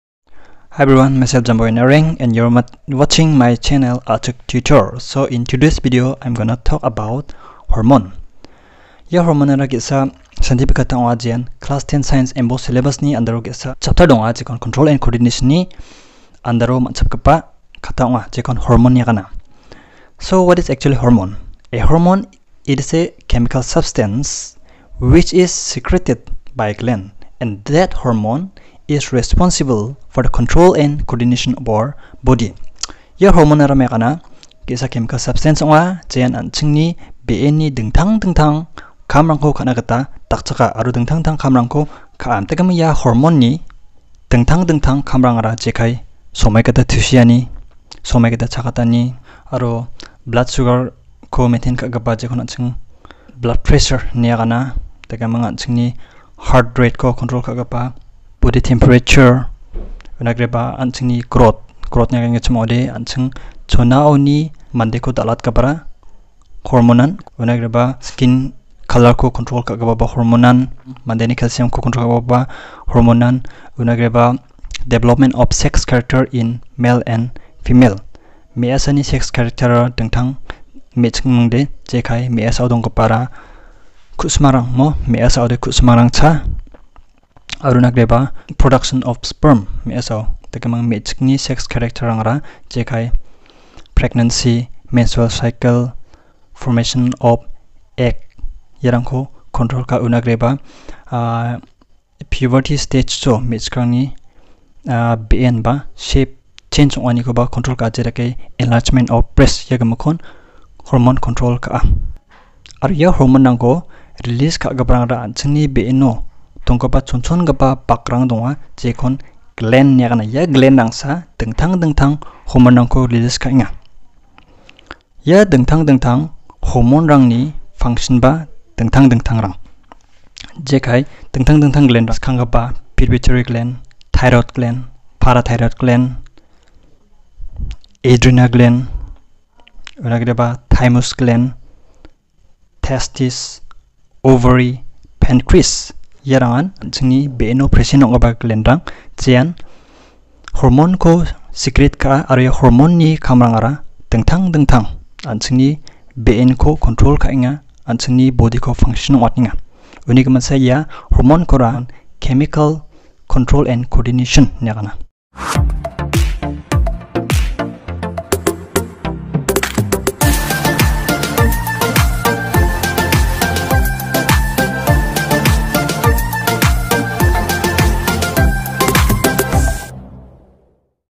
What is Hormone? || Class 10 Science || Control and coordination || Garo lecture.
Hello friends I've made this video on Hormone which is related to control and coordination chapter of Class x MBOSE.

Mitela 🙏